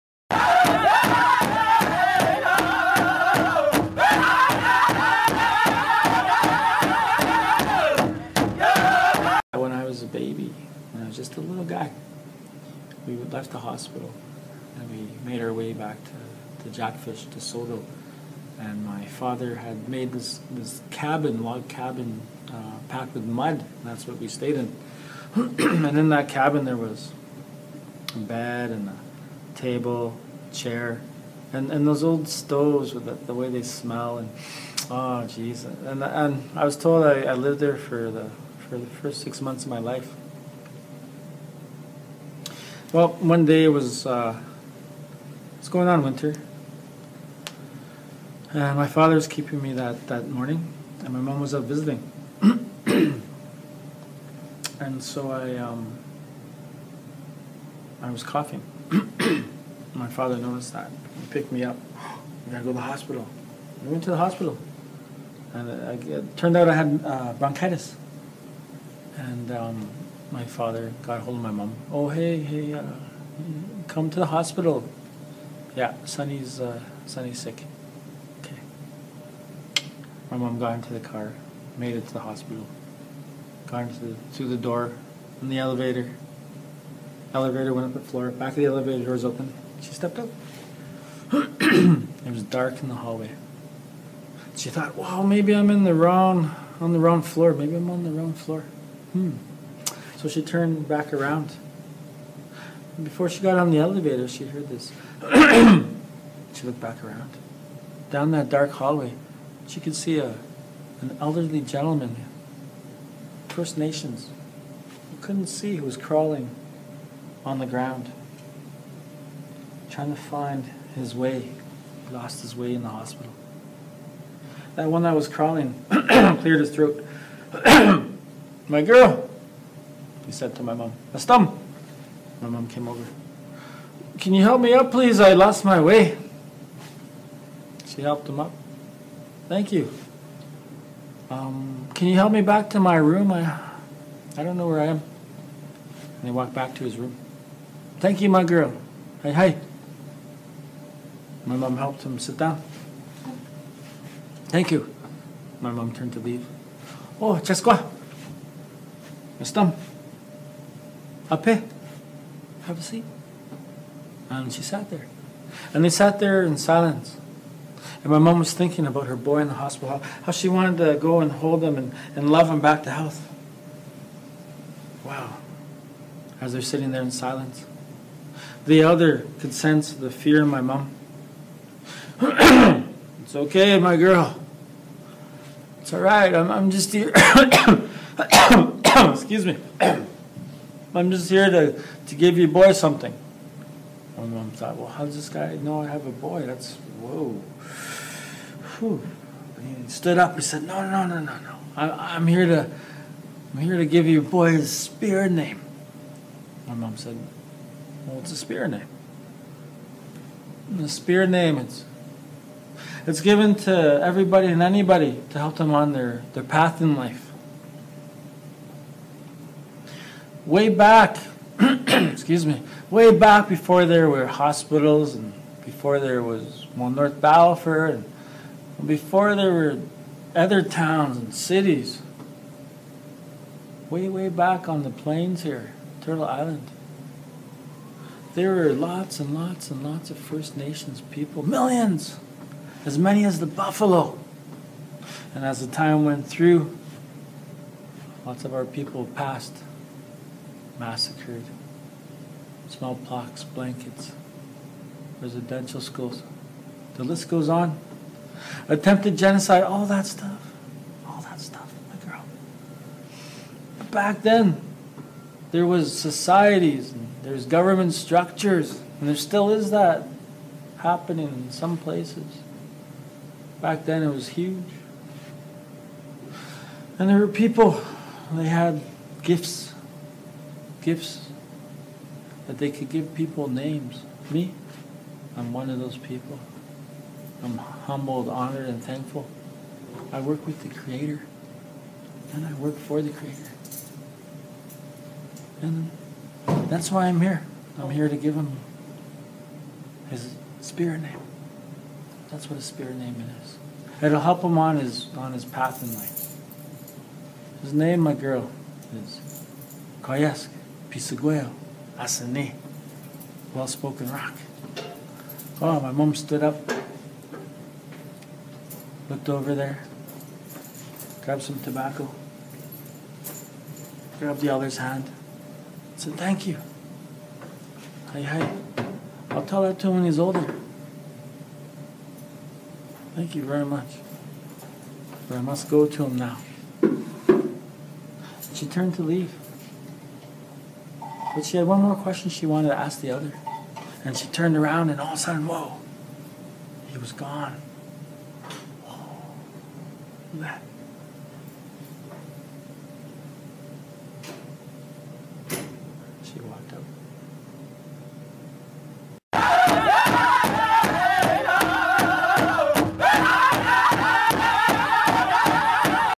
Well Spoken Rock
Visual depiction of Simon Moccasin's naming story.